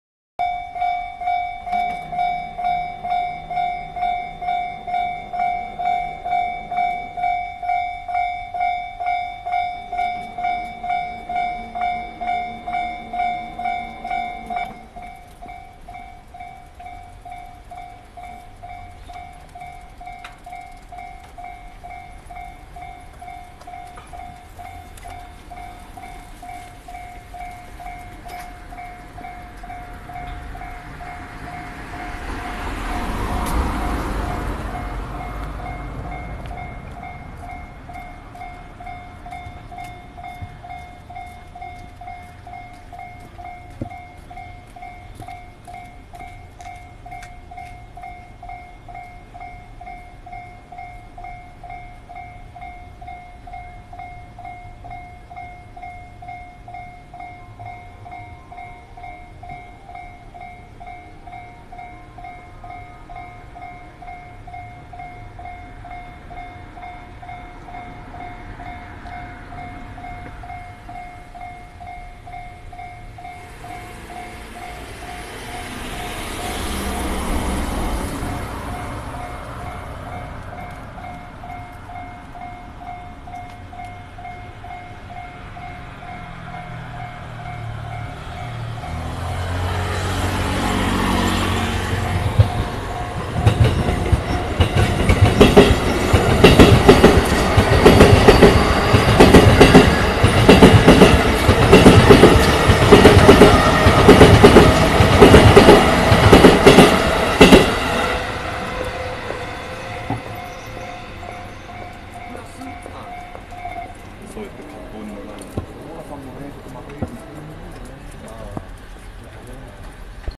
JR中央線 八王子街道踏切 その1
JR中央線　八王子～豊田間　4ヶ所目の踏切です！この踏切は､豊田駅付近にあります！（2015年10月に撮影した物です！）